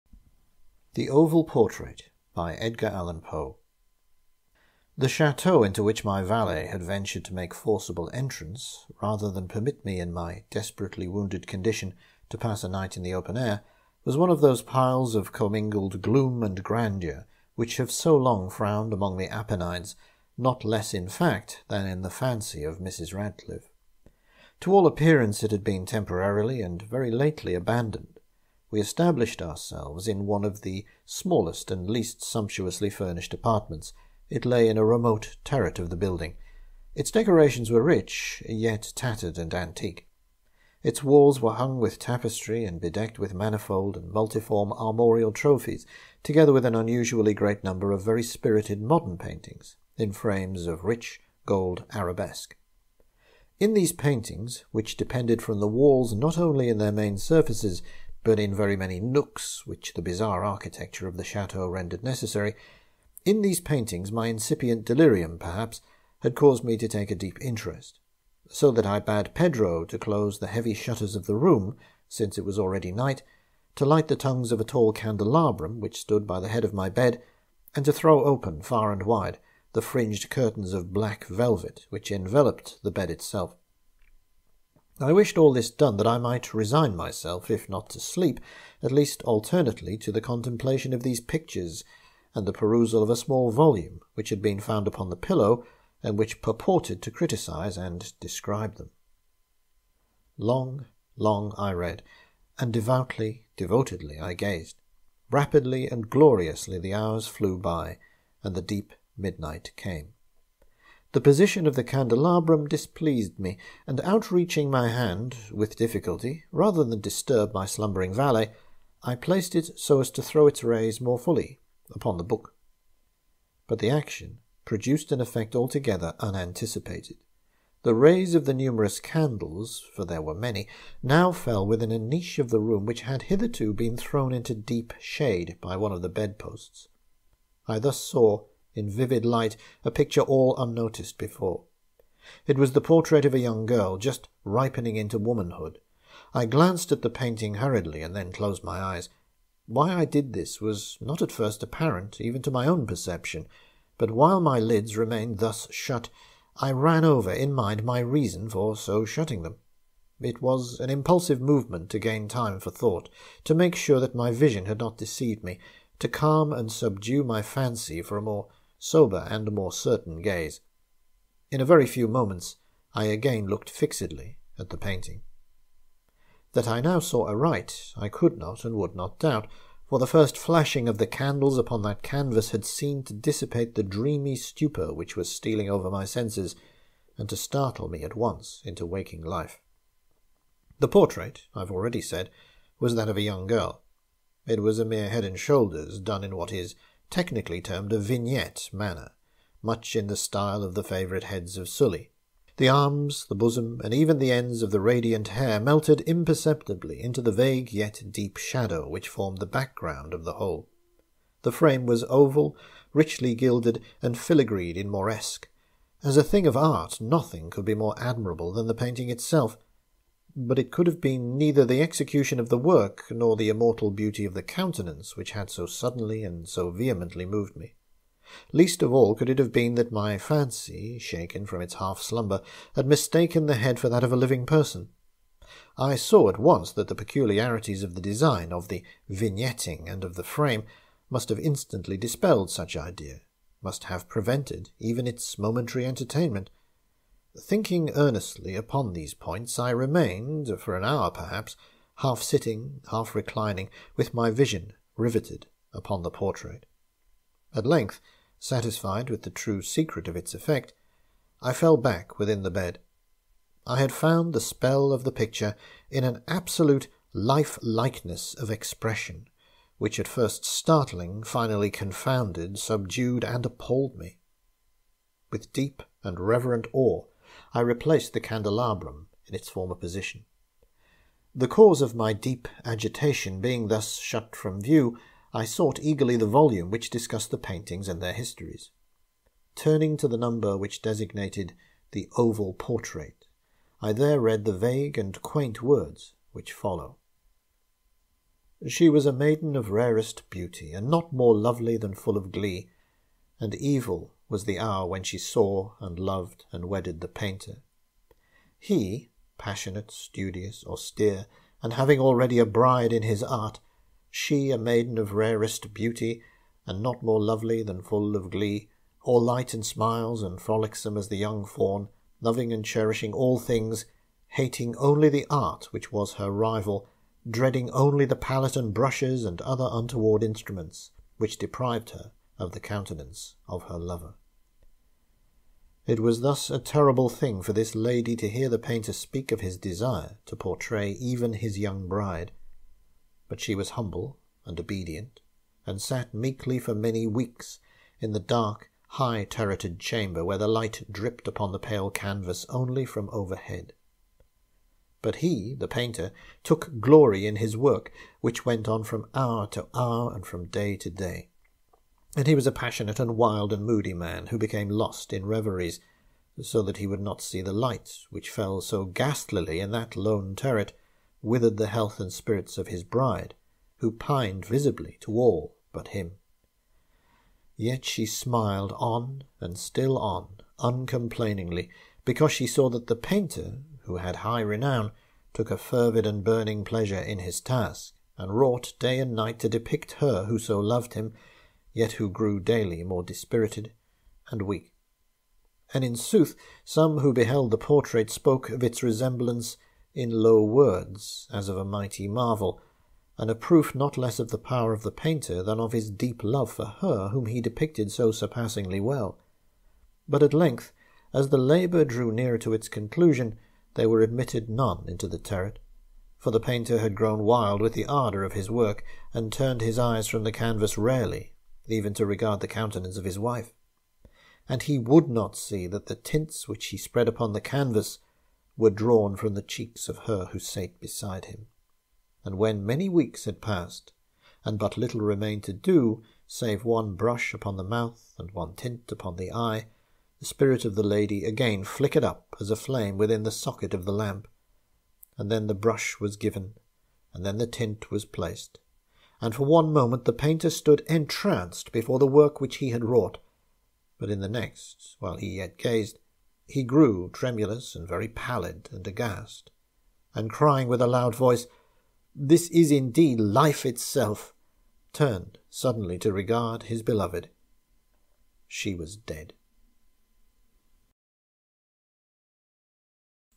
'The Oval Portrait' by Edgar Allan Poe
Read by David Longhorn